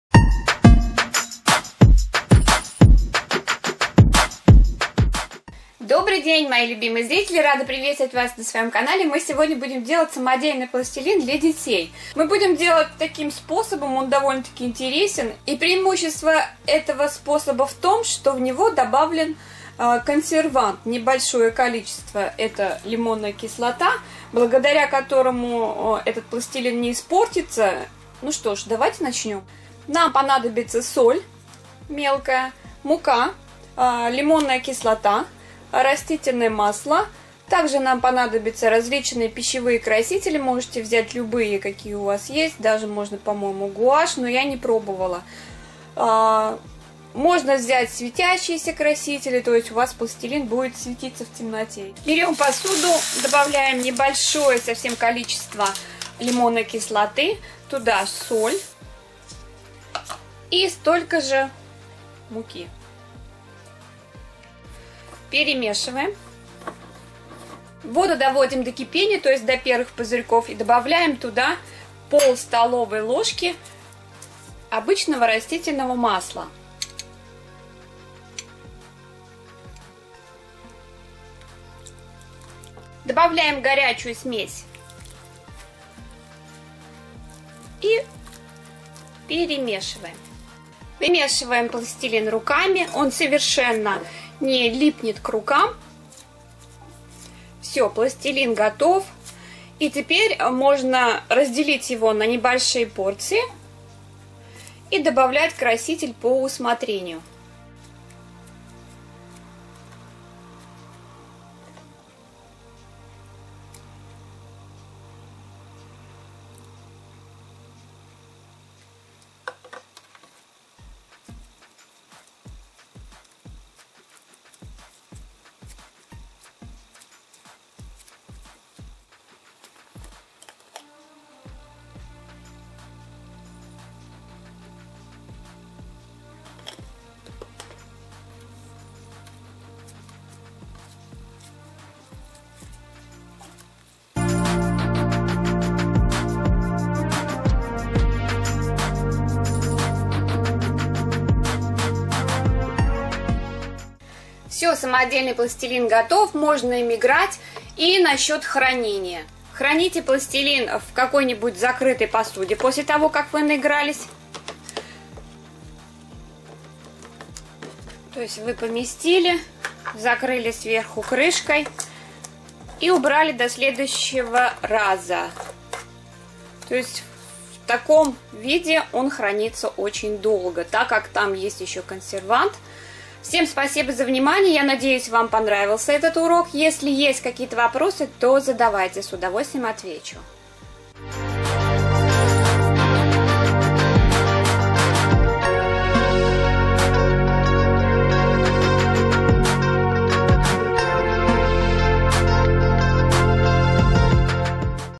DIY ПЛАСТИЛИН своими РУКАМИ♡♡♡БЫСТРО, ПРОСТО, БЕЗОПАСНО√Elena Matveeva
Классный способ приготовить в домашних условиях пластилин для детей (и не только...), он совершенно безопасен, хранится долгое время и очень пластичен!
✽Обязательно делитесь видео с друзьями!!! Они ооочень подбадривают и хочется продолжать вести канал:))) Elena Matveeva:)